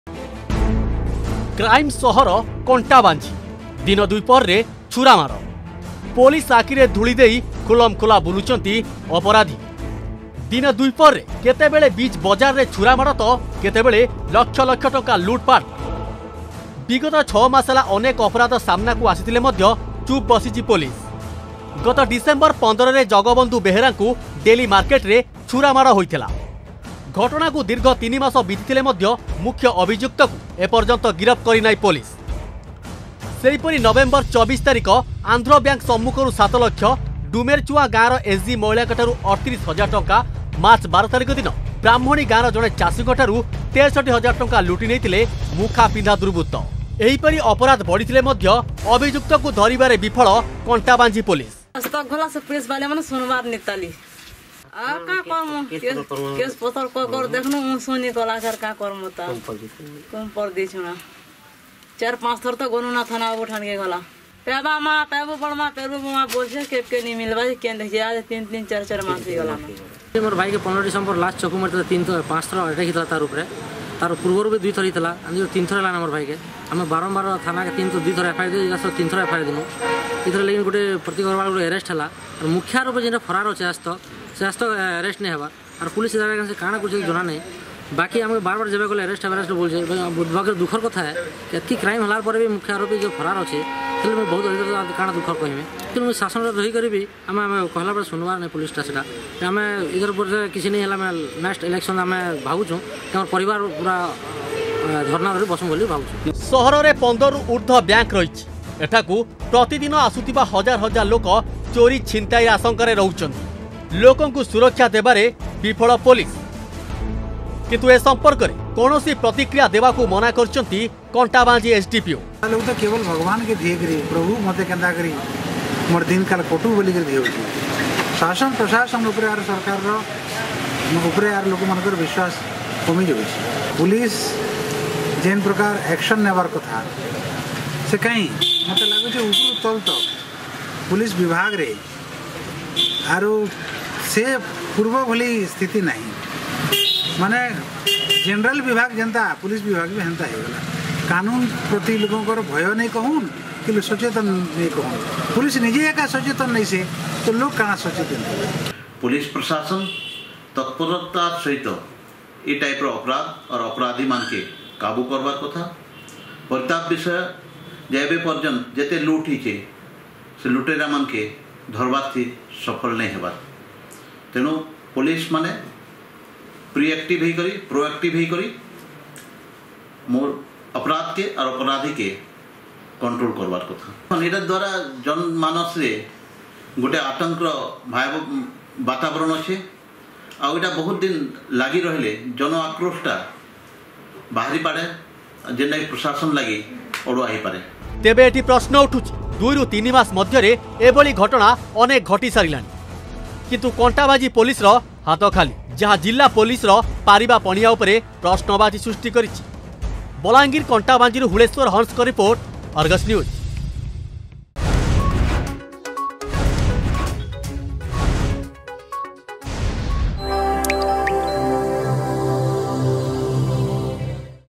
କ୍ରାଇମ ସହର କଣ୍ଟାବାଞ୍ଜି...
ଦିନ ଦ୍ଵିପହରରେ କେତେବେଳେ ବିଚ୍ ବଜାରରେ ଛୁରା ମାଡ଼ ତ କେତେବେଳେ ଲକ୍ଷ ଲକ୍ଷ ଟଙ୍କା ଲୁଟ୍ ପାଟ୍ । ବିଗତ 6 ମାସ ହେଲା ଅନେକ ଅପରାଧ ସାମ୍ନାକୁ ଆସିଥିଲେ ମଧ୍ୟ ଚୁପ୍ ପୋଲିସ.